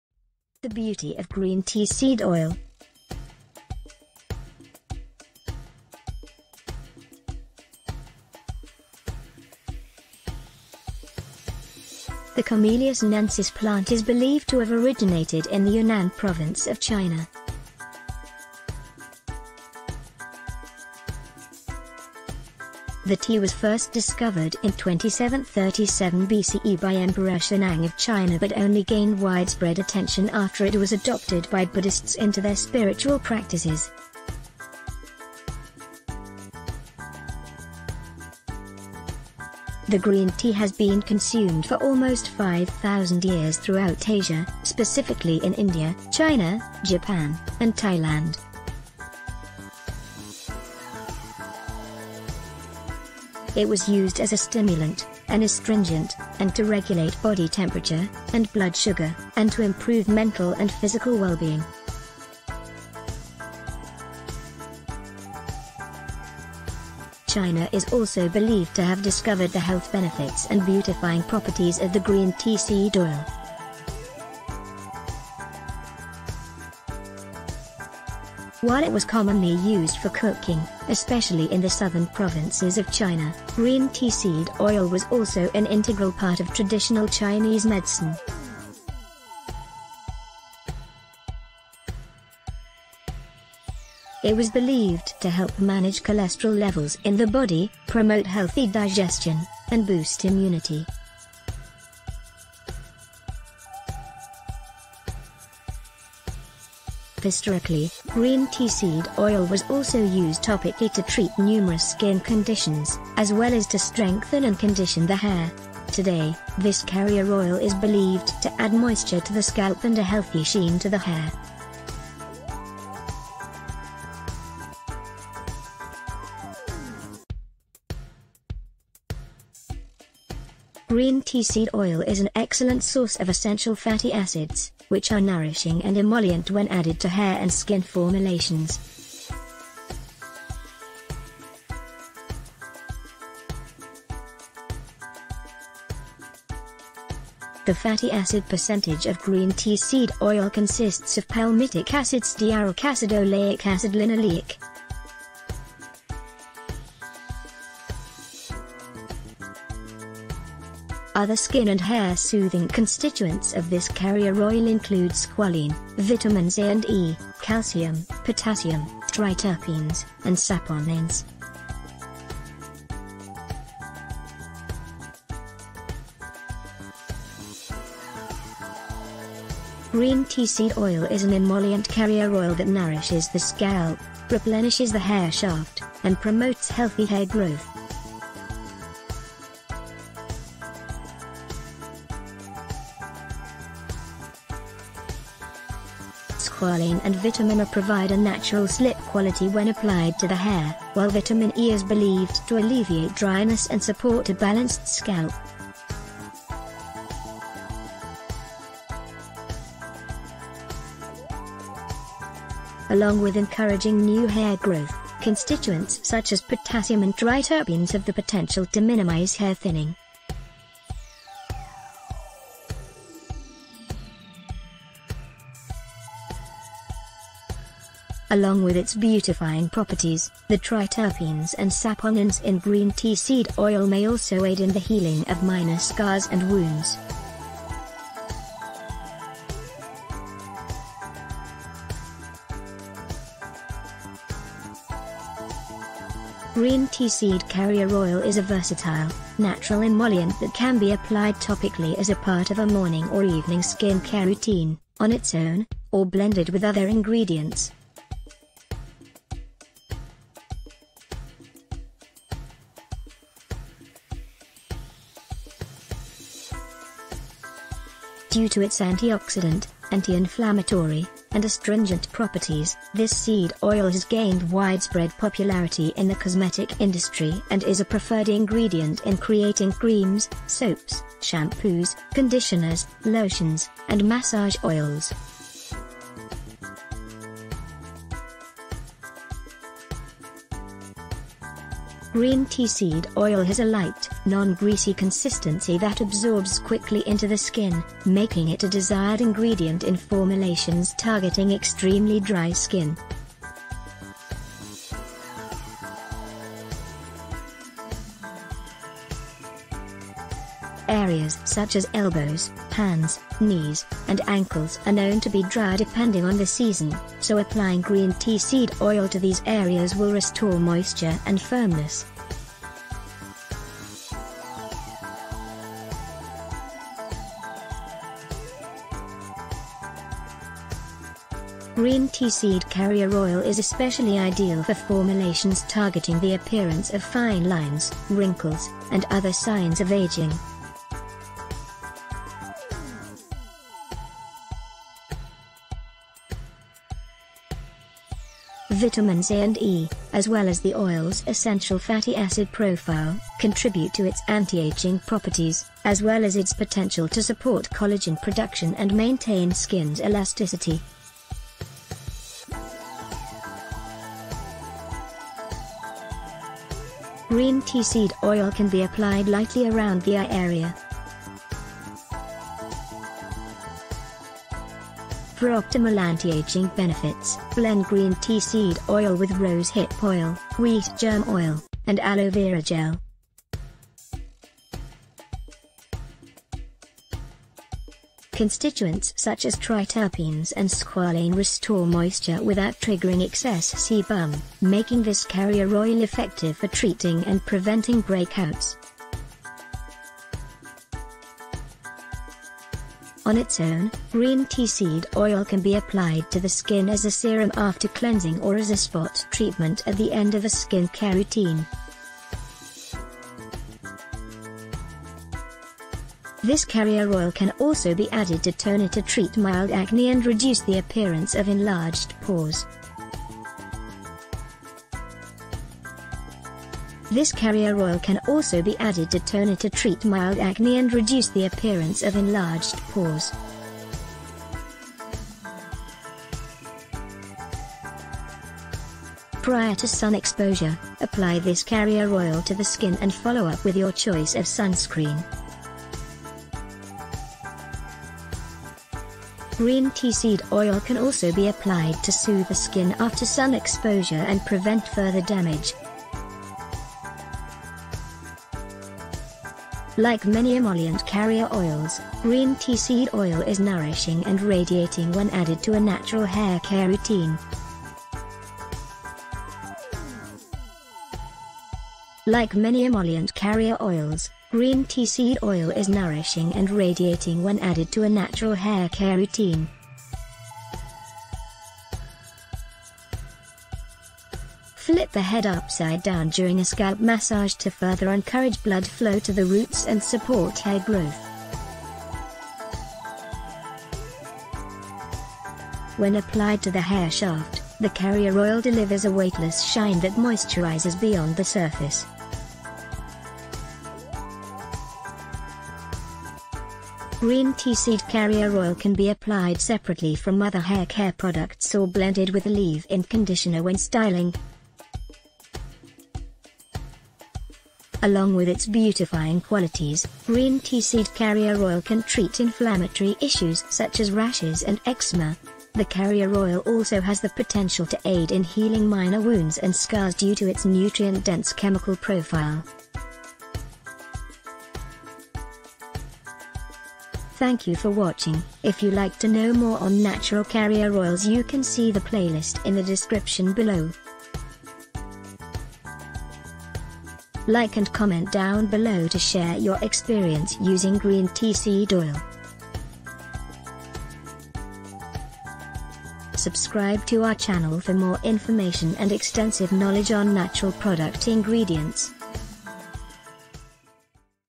THE BEAUTY OF GREEN TEA SEED OIL AND THE WONDERFUL BENEFIT FOR YOUR SKIN
Green Tea Seed Oil is a light, non-greasy carrier oil that is quickly absorbed by the skin. It is rich in vitamins, minerals, and antioxidants, making it beneficial for an array of skin types, including acne-prone, aging, dry, and combination skin.
Green Tea Seed Oil is derived from the seeds of Camellia sinensis, a Tea plant that is native to Southeast Asia and a part of the Theaceae family.
Two of the most common species of the Camellia plant are Camellia sinensis and Camellia oleifera.
The leaves of the Tea plant are dried to produce black tea, oolong tea, and green tea. Meanwhile, the seeds are cold-pressed to extract a concentrated oil that possesses a high level of fatty acids as well as many of the same minerals and antioxidants found in tea.
The chemical profile of Green Tea Seed Carrier Oil includes over 80% Oleic Acid, Linoleic Acid, Alpha-Linolenic Acid, Stearic Acid, Vitamin E, Calcium, Potassium, Triterpens, Saponins, and Squalane.